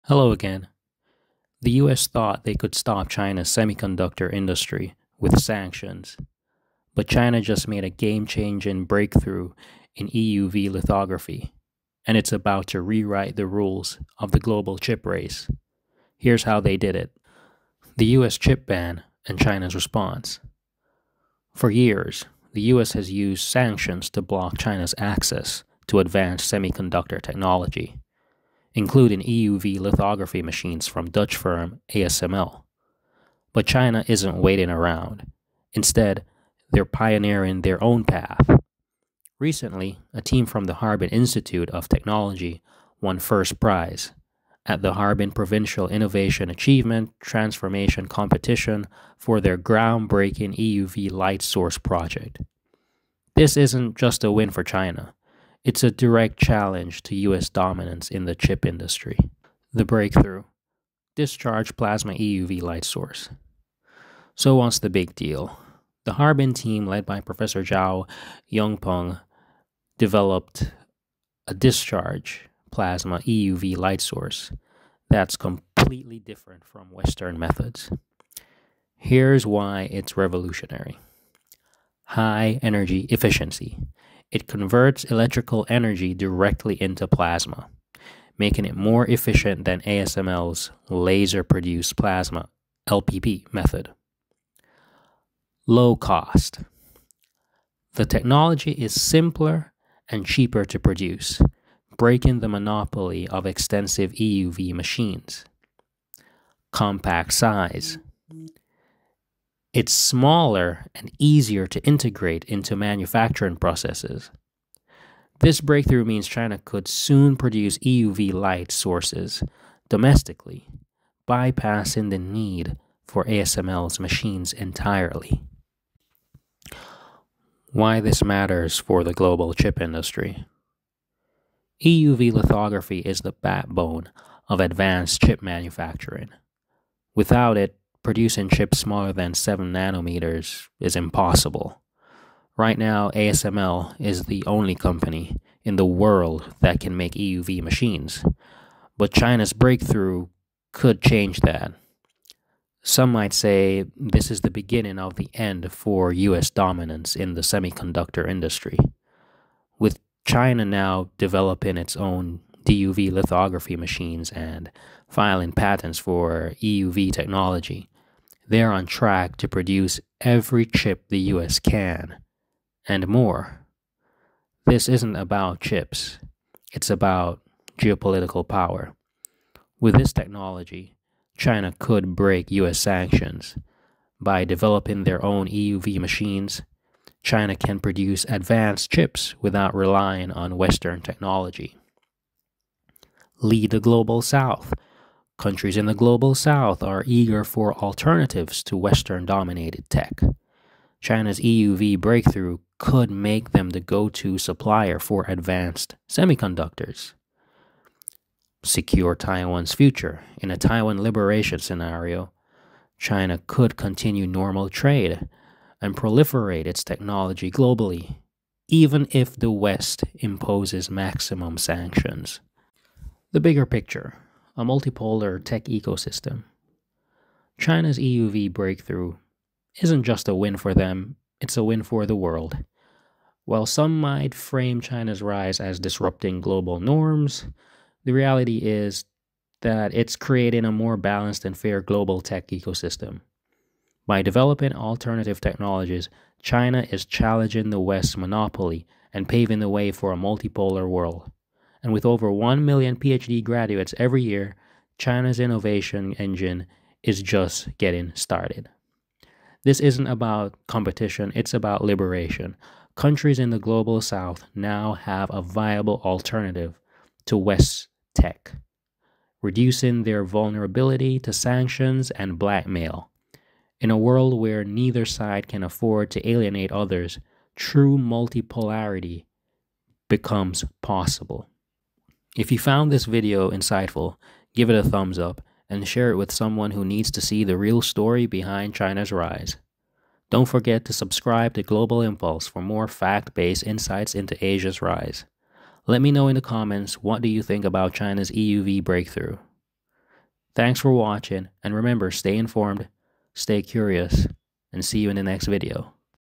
China Just Beat the US Chip Ban with EUV breakthrough (ASML Worried!)
The U.S. thought they could stop China’s semiconductor industry with sanctions. But China just made a game-changing breakthrough in EUV lithography—and it’s about to rewrite the rules of the global chip race.

In this video, we dive into China’s latest EUV breakthrough, led by Professor Zhao Yongpeng and his team at the Harbin Institute of Technology. Discover how their discharge plasma EUV light source could sidestep the U.S. chip ban, challenge ASML’s monopoly, and reshape the global semiconductor industry.

From breaking U.S. sanctions to leading the Global South, China’s EUV breakthrough isn’t just about chips—it’s about geopolitical power and creating a fairer, multipolar world.